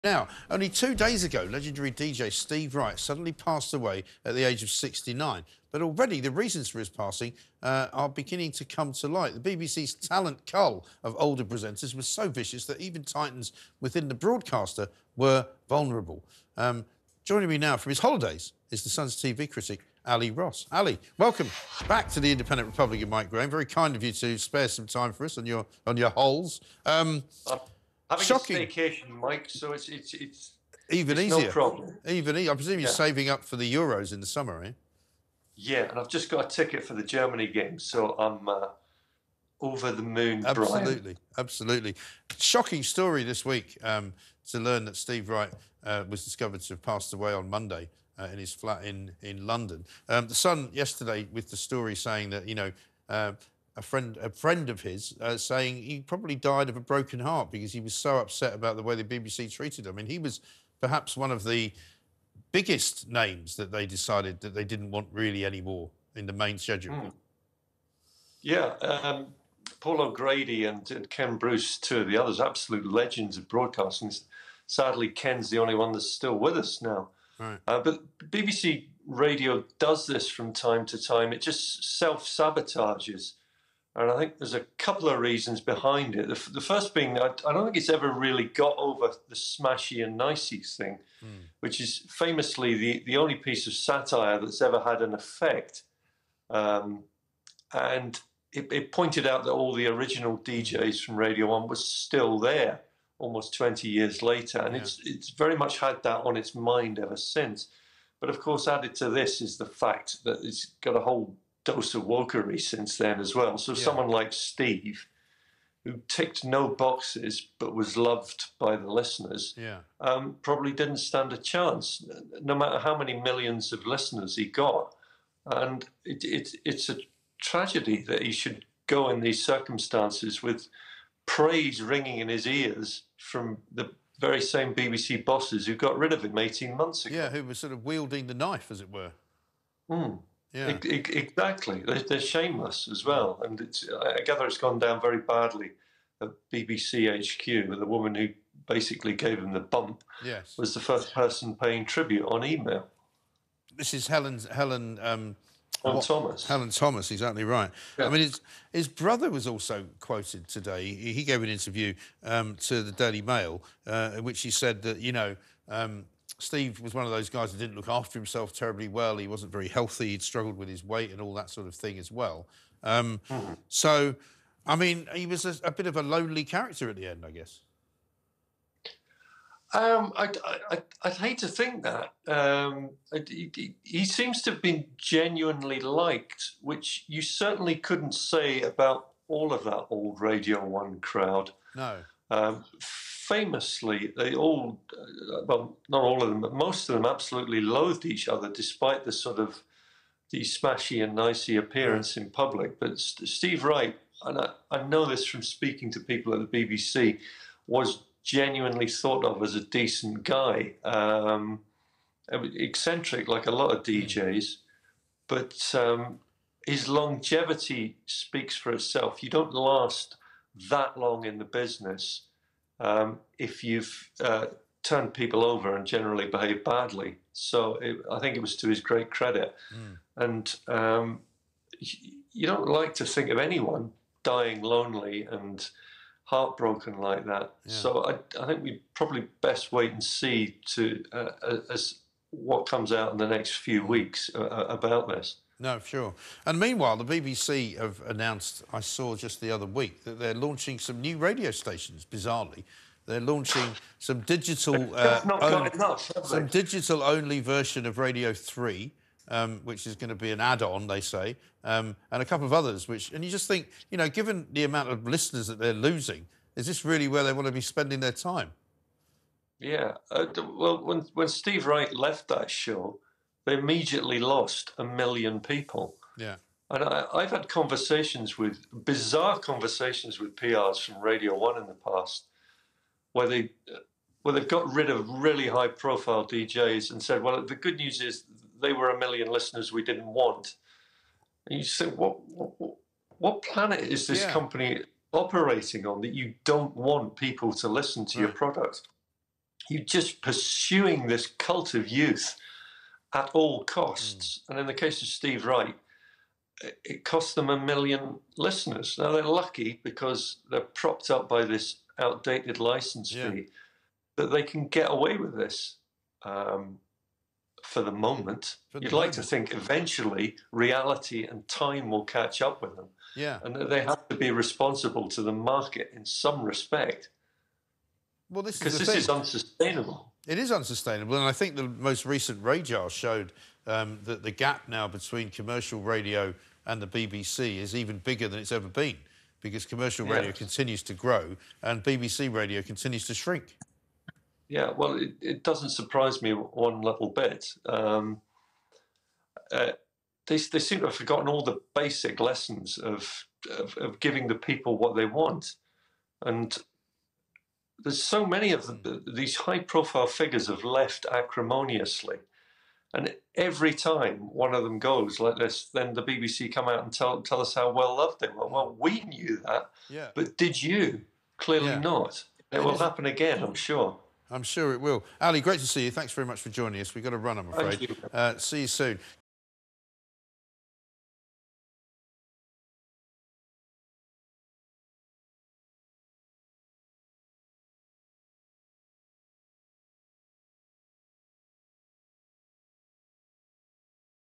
Steve Wright Probably Died "Of A Broken Heart" According To Friends
Steve Wright, the radio legend, is said to have "died of a broken heart" after being let go by the BBC, according to friends.

He struggled to cope with being removed from his Radio 2 afternoon show, which he had hosted for 24 years.

The decision to end his popular show in 2022 sparked backlash against the BBC boss responsible for the changes. Helen Thomas, in a tribute, described Steve as "second to none."

Steve, aged 69, was found dead in his London flat on Monday, and his death is not considered suspicious.

Mike Graham speaks with The Sun's TV critic Ally Ross on the backlash the BBC have faced since the DJ's passing.